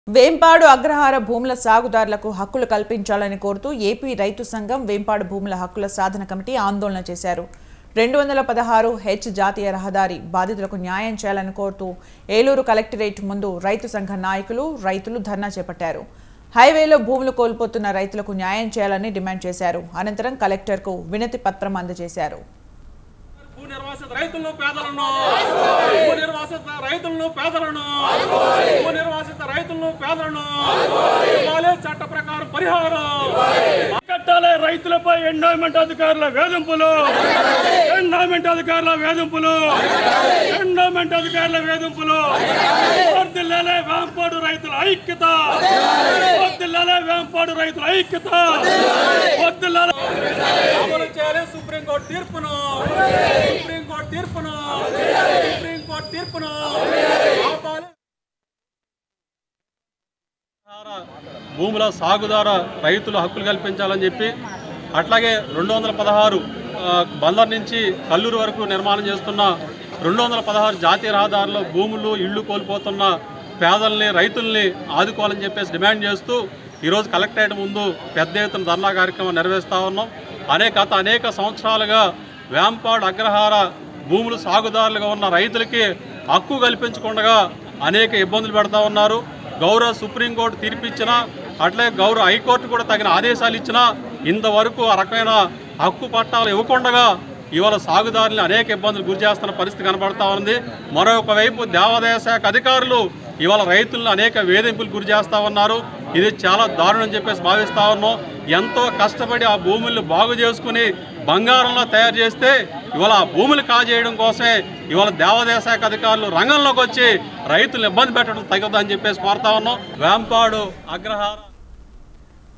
ఏలూరు కలెక్టరేట్ ముందు రైతుల ధర్నా | Farmers Protest in front of Eluru Collectorate | Swatantra TV
హైవే లో భూములు కోల్పోతున్న రైతులకు న్యాయం చెయ్యాలని డిమాండ్